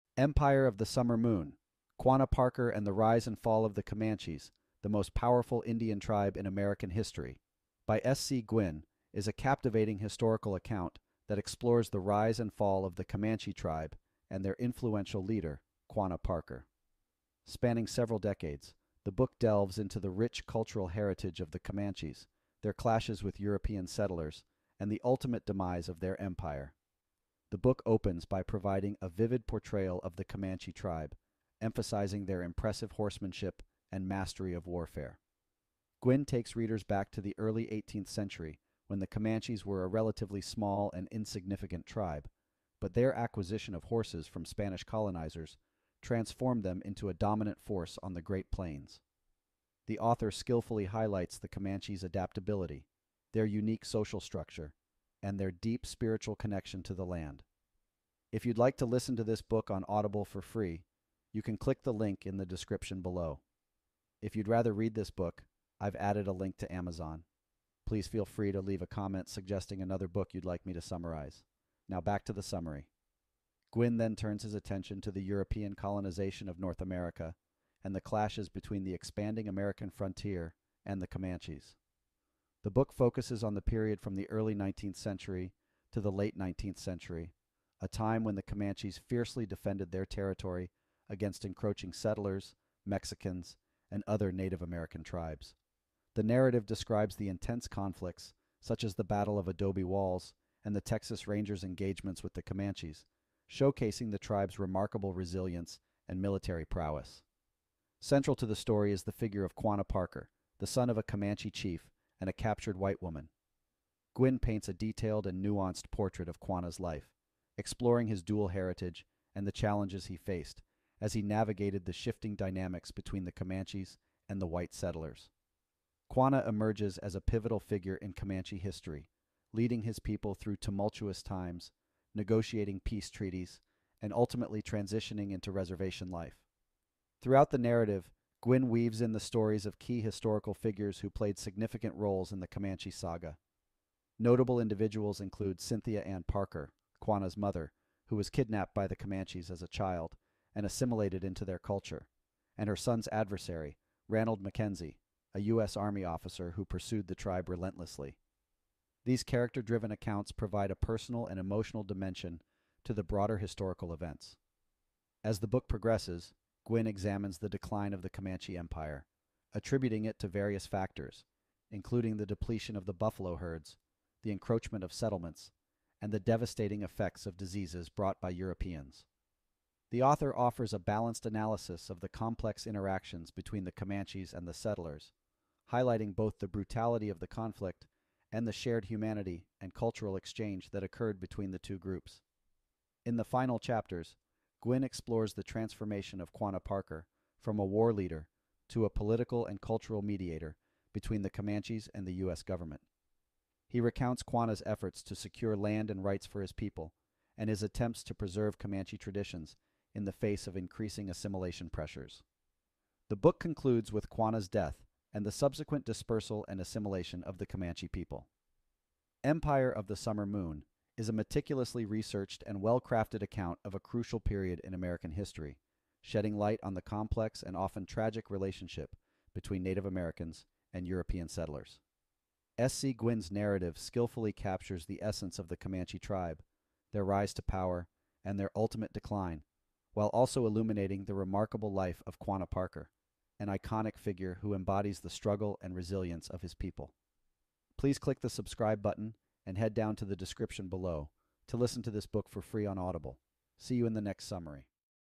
Empire of the Summer Moon by S. C. Gwynne
This is a quick summary of Empire of the Summer Moon by S. C. Gwynne.

What Amazon says:
Empire of the Summer Moon spans two astonishing stories. The first traces the rise and fall of the Comanches, the most powerful Indian tribe in American history. The second entails one of the most remarkable narratives ever to come out of the Old West: the epic saga of the pioneer woman Cynthia Ann Parker and her mixed-blood son, Quanah, who became the last and greatest chief of the Comanches.

Although listeners may be more familiar with the names Apache and Sioux, it was in fact the legendary fighting ability of the Comanches that determined just how and when the American West opened up. They were so masterful at war and so skillful with their arrows and lances that they stopped the northern drive of colonial Spain from Mexico and halted the French expansion westward from Louisiana. White settlers arriving in Texas from the Eastern United States were surprised to find the frontier being rolled backward by Comanches incensed by the invasion of their tribal lands. So effective were the Comanches that they forced the creation of the Texas Rangers and account for the advent of the new weapon specifically designed to fight them: the six-gun.

The war with the Comanches lasted four decades, in effect holding up the development of the new American nation. Gwynne's exhilarating account delivers a sweeping narrative that encompasses Spanish colonialism, the Civil War, the destruction of the buffalo herds, and the arrival of the railroads - a historical feast for anyone interested in how the United States came into being.

The Summary:
"Empire of the Summer Moon: Quanah Parker and the Rise and Fall of the Comanches, the Most Powerful Indian Tribe in American History" by S. C. Gwynne is a captivating historical account that explores the rise and fall of the Comanche tribe and their influential leader, Quanah Parker. Spanning several decades, the book delves into the rich cultural heritage of the Comanches, their clashes with European settlers, and the ultimate demise of their empire.
The book opens by providing a vivid portrayal of the Comanche tribe, emphasizing their impressive horsemanship and mastery of warfare. Gwynne takes readers back to the early 18th century when the Comanches were a relatively small and insignificant tribe, but their acquisition of horses from Spanish colonizers transformed them into a dominant force on the Great Plains. The author skillfully highlights the Comanches' adaptability, their unique social structure, and their deep spiritual connection to the land. If you'd rather read this book I've added a link to Amazon. Now back to the summary.
Gwynne then turns his attention to the European colonization of North America and the clashes between the expanding American frontier and the Comanches. The book focuses on the period from the early 19th century to the late 19th century, a time when the Comanches fiercely defended their territory against encroaching settlers, Mexicans, and other Native American tribes. The narrative describes the intense conflicts, such as the Battle of Adobe Walls and the Texas Rangers' engagements with the Comanches, showcasing the tribe's remarkable resilience and military prowess.
Central to the story is the figure of Quanah Parker, the son of a Comanche chief and a captured white woman. Gwynne paints a detailed and nuanced portrait of Quanah's life, exploring his dual heritage and the challenges he faced as he navigated the shifting dynamics between the Comanches and the white settlers. Quanah emerges as a pivotal figure in Comanche history, leading his people through tumultuous times, negotiating peace treaties, and ultimately transitioning into reservation life.
Throughout the narrative, Gwynne weaves in the stories of key historical figures who played significant roles in the Comanche saga. Notable individuals include Cynthia Ann Parker, Quanah's mother, who was kidnapped by the Comanches as a child and assimilated into their culture, and her son's adversary, Ranald Mackenzie, a U.S. Army officer who pursued the tribe relentlessly. These character-driven accounts provide a personal and emotional dimension to the broader historical events.
As the book progresses, Gwynne examines the decline of the Comanche empire, attributing it to various factors, including the depletion of the buffalo herds, the encroachment of settlements, and the devastating effects of diseases brought by Europeans.